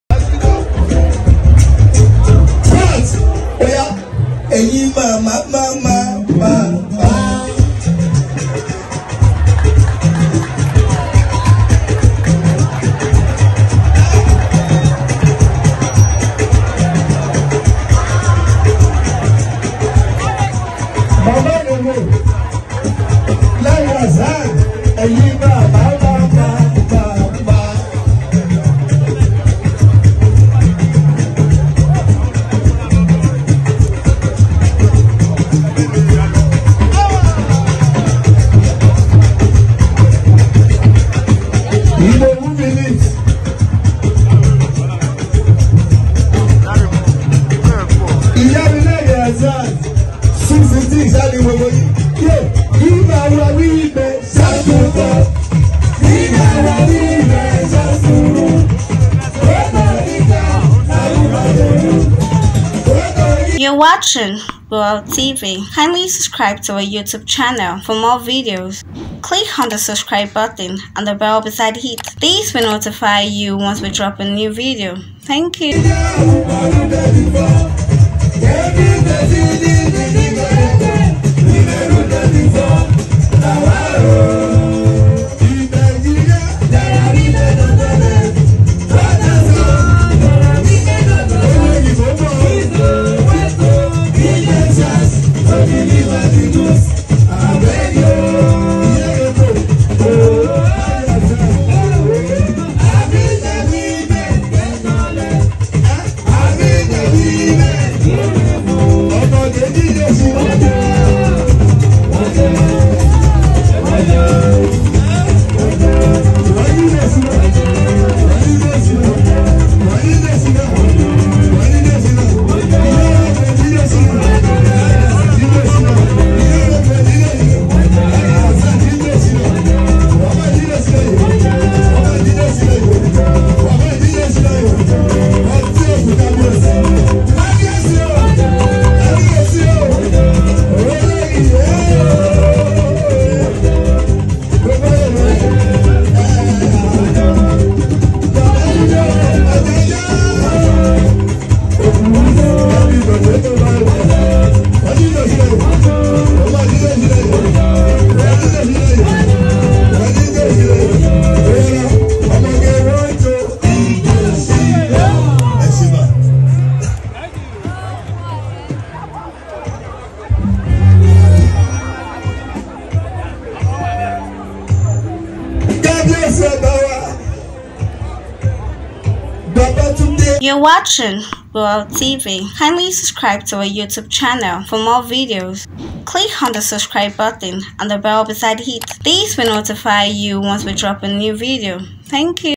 Iya Rainbow,Mama Awero Shocks Everyone As They Dance For Ooni Of Ife &His Wife As Shina Peters Sings
Iya Rainbow,Mama Awero Shocks Everyone As They Dance For Ooni Of Ife & His Wife & Shina Peters Preform